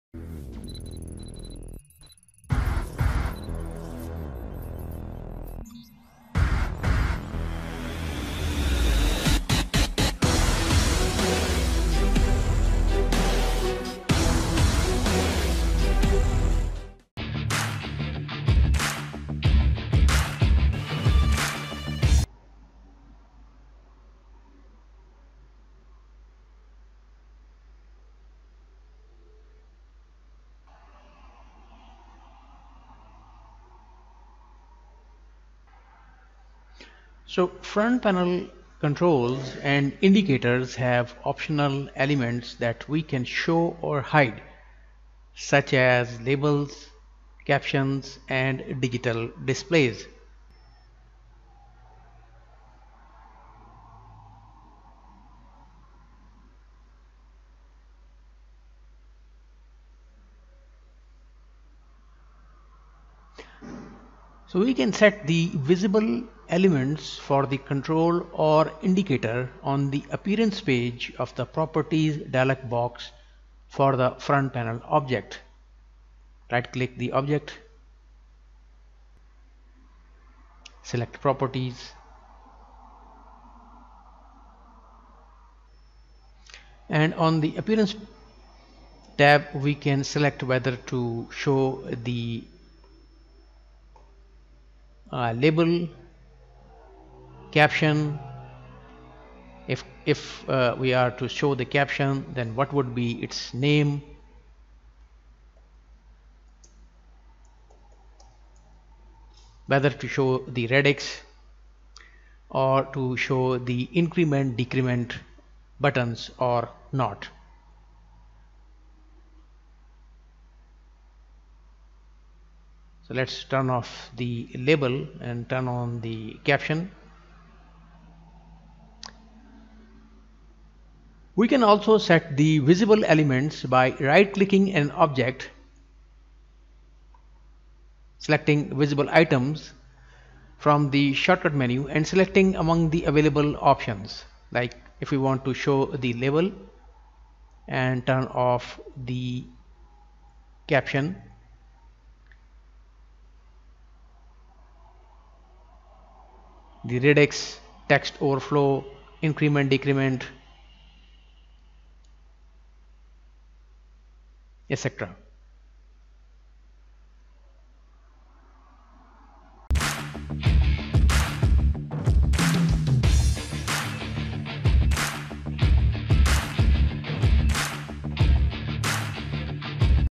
Showing and Hiding Optional Front Panel Elements in LabVIEW
Front panel controls and indicators have optional elements we can show or hide, such as labels, captions, and digital displays. Set the visible elements for the control or indicator on the Appearance page of the Properties dialog box for the front panel object. We can also set the visible elements by right-clicking an object, selecting Visible Items from the shortcut menu, and selecting among the available options.